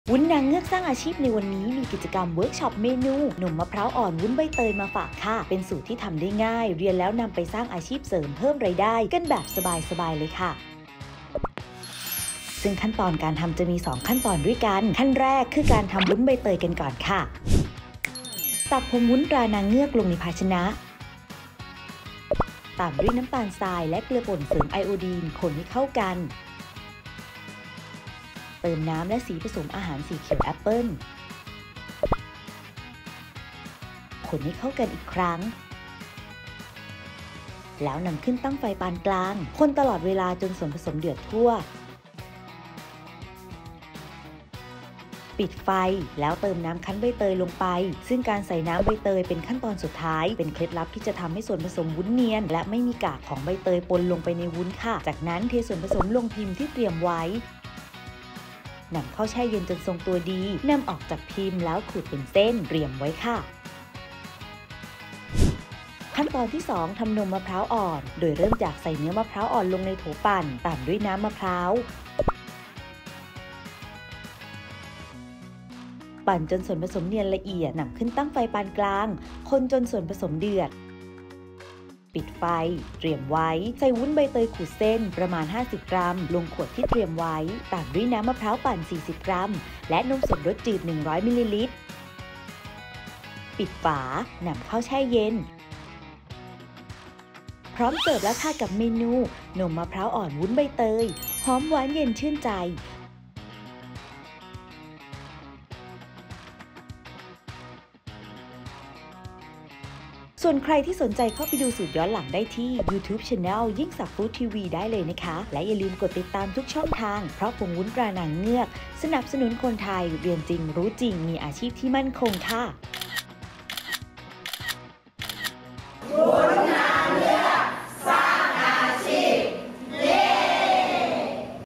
"นมมะพร้าวอ่อนวุ้นใบเตย" วุ้นนางเงือก สร้างอาชีพ สูตรเด็ดอ.ยิ่งศักดิ์ ทำง่ายอร่อยด้วย | Cooking Guru
ใครที่อยากมีอาชีพเสริมเพิ่มรายได้ ห้ามพลาดวุ้นนางเงือกสร้างอาชีพ จัดกิจกรรมเวิร์คช้อปดีดีเรียนแล้วได้สูตรวุ้นอร่อยๆ เอาไว้ทำขายกำไรดีแน่นอน วันนี้มากับเมนู นมมะพร้าวอ่อนวุ้นใบเตย ทำง่ายอร่อยด้วย

▲Line @yingsakfood